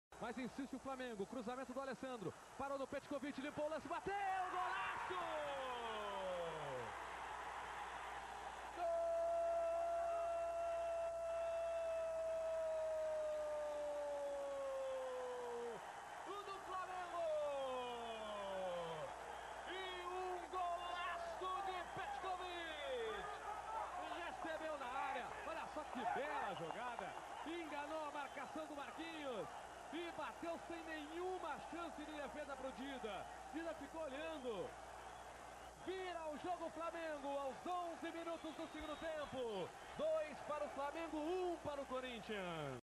GOLAÇO - Petkovic - Flamengo x Corinthians [14/10/2001] (0000)-
Um golaço do ídolo do Flamengo se livrando dos marcadores e chutando no canto do goleiro.